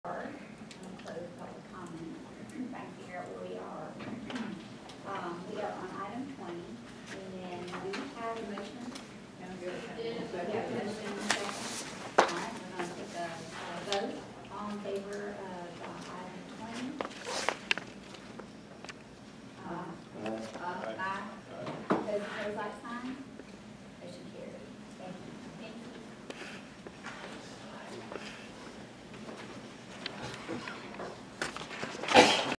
Approved the Parker Pond permit unanimously. --Board @ SRWMD 2019-05-14
Parker Pond water withdrawals, agriculture, and springs @ SRWMD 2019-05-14